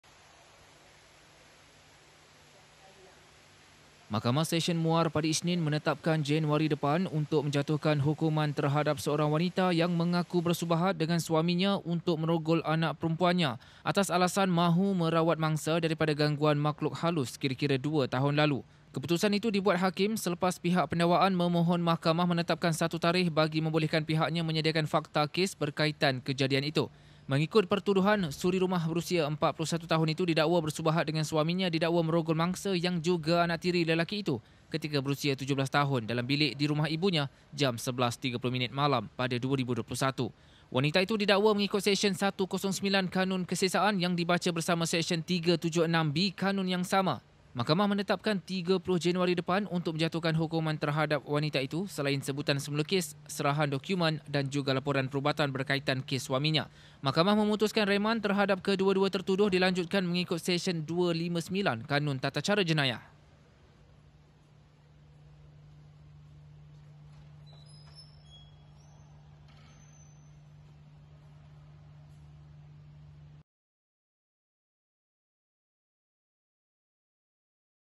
Ibu mengaku subahat, suami rogol anak tiri
MUAR: Mahkamah Sesyen di sini hari ini, menetapkan Januari depan untuk menjatuhkan hukuman terhadap seorang ibu yang mengaku salah, bersubahat dengan suami merogol anak kandung, kononnya mahu merawat mangsa diganggu makhluk halus, dua tahun lalu.